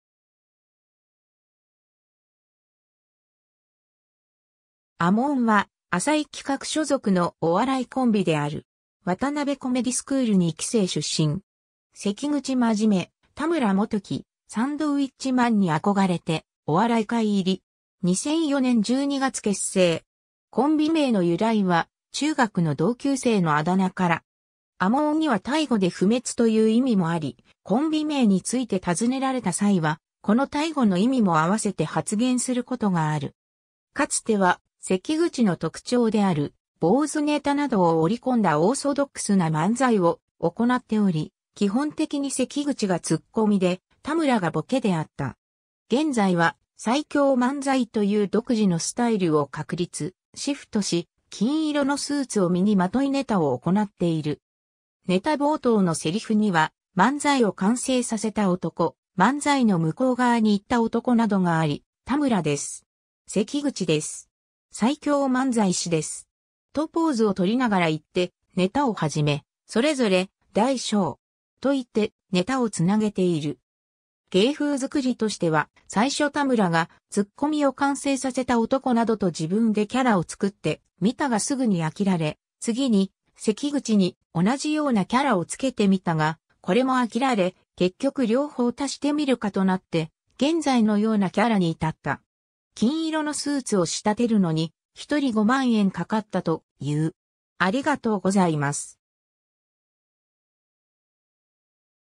アモーン
アモーンは、浅井企画所属のお笑いコンビである。
ワタナベコメディスクール2期生出身。
 関口 まじめ（せきぐち まじめ　 (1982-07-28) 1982年7月28日（39歳） - ） 田村 元樹（たむら もとき　 (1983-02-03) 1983年2月3日（38歳） - ） サンドウィッチマンに憧れてお笑い界入り。
2004年12月結成。
コンビ名の由来は、中学の同級生のあだ名から。
アモーンにはタイ語で不滅という意味もあり、コンビ名について尋ねられた際は、このタイ語の意味も併せて発言することがある。
 かつては関口の特徴である坊主ネタなどを織り込んだオーソドックスな漫才を行っており、基本的に関口がツッコミで田村がボケであった。
現在は最強漫才という独自のスタイルを確立、シフトし、金色のスーツを身にまといネタを行っている。
ネタ冒頭の台詞には「漫才を完成させた男」（田村）、「漫才の向こう側に行った男」（関口）などがあり、「田村です！」「関口です！」「最強漫才師です！」とポーズをとりながら言ってネタを始め、それぞれ「第○章！」と言ってネタを繋げている。
芸風作りとしては、最初田村が「ツッコミを完成させた男」などと自分でキャラを作ってみたがすぐに飽きられ、次に関口に同じようなキャラを付けてみたがこれも飽きられ、結局両方足してみるかとなって現在のようなキャラに至った。
金色のスーツを仕立てるのに、一人5万円かかったという。